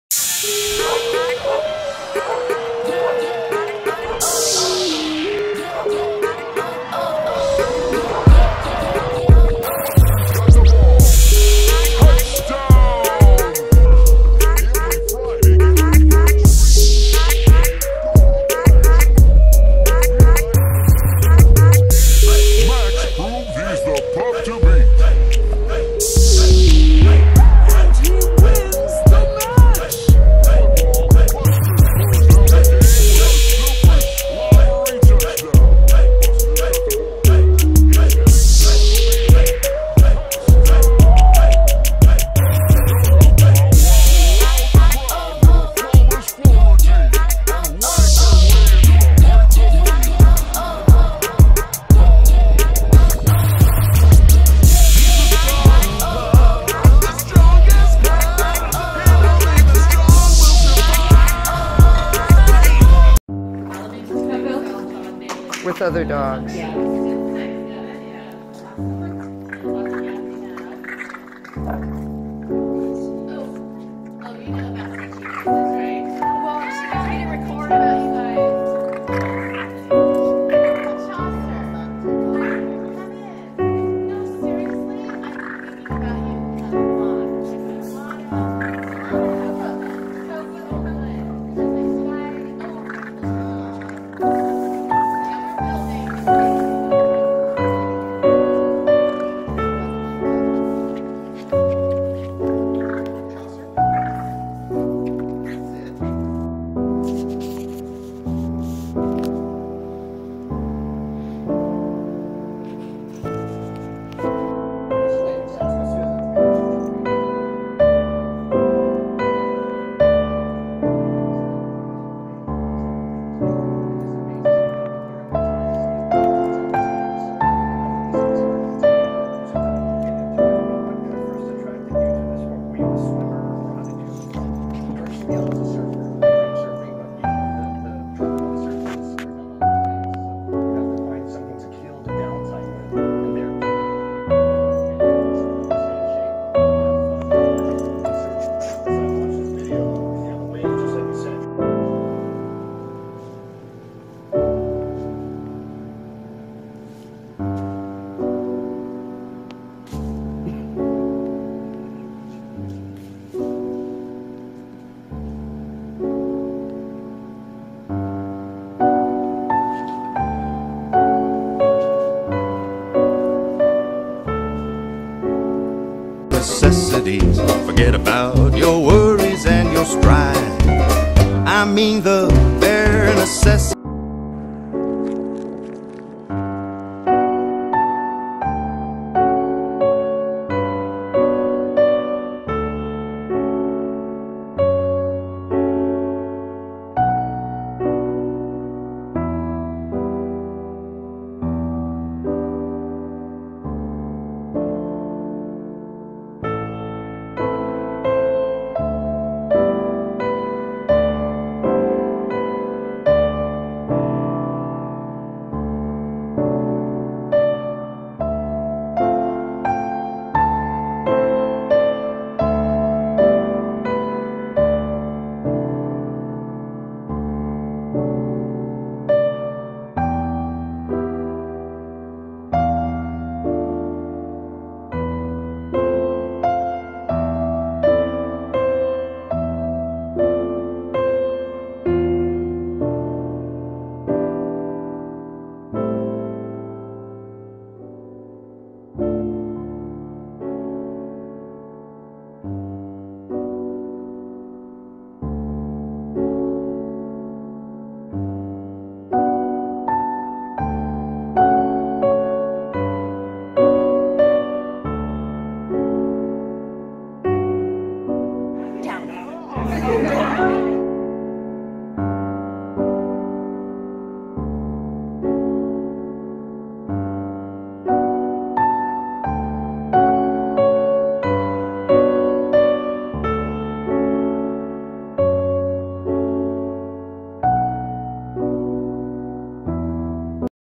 Funny Dogs Sports And Playing Compilation
This video is the compilation of amazing dog playing sports, funny and friendly cats playing compilation. I hope you will enjoy it. They are cute and adorable pets to watch.

Video Title: Franklin’s tail sure looks like a toy to me
Video Title: My Best Friend @ Smartypup! Puppy Socials
Video Title: SmartyPup! Social Perfection.
Video Title: Super Snuggly Sports Spectacular - Strong Pup